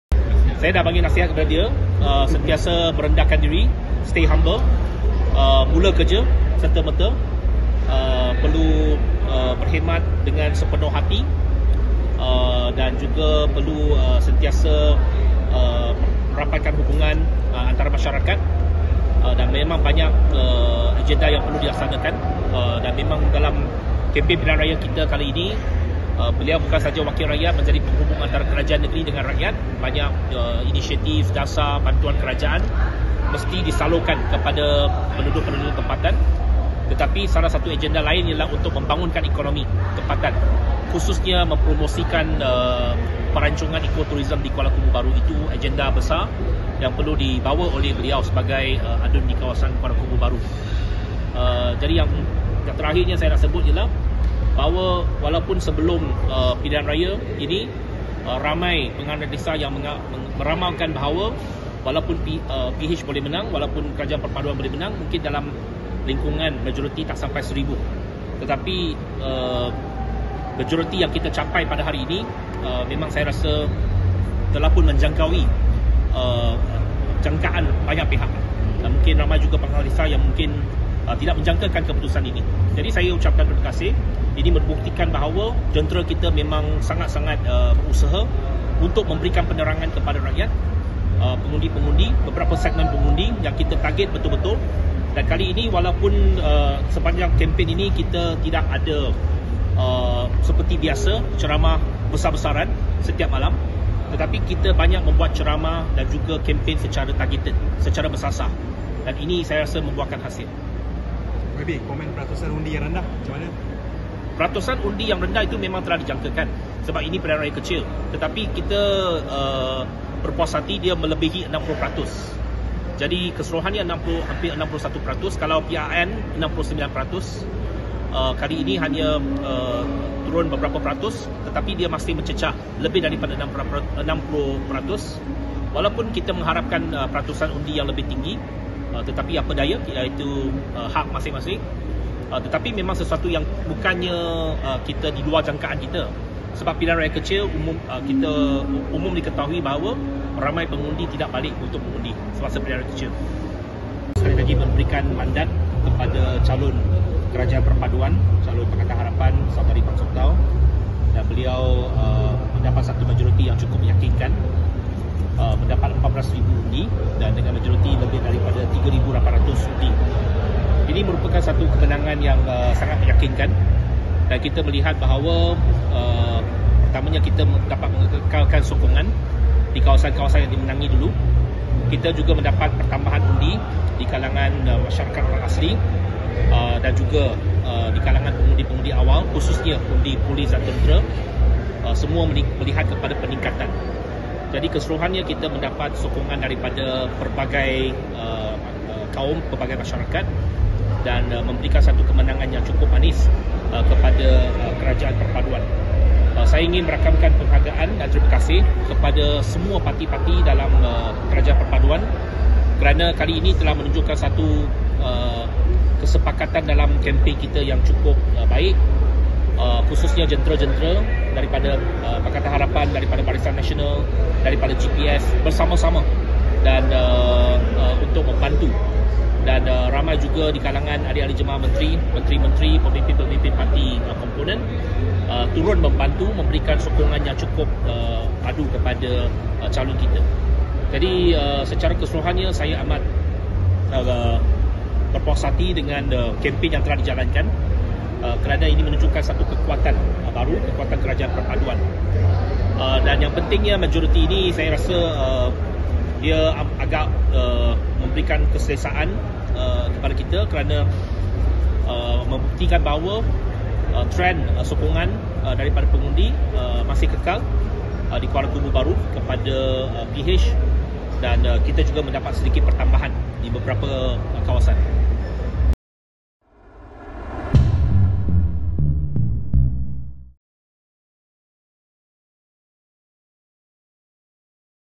Kuala Kubu Baharu victory shows unity government's strength, says Loke
DAP secretary-general Anthony Loke believes that Pakatan Harapan's victory in the Kuala Khubu Baharu by-election signals the unity government’s strength.

Speaking to the media after attending PKR 25th Anniversary celebrations, Loke said that the result proves that KKB voters are still with Pakatan Harapan.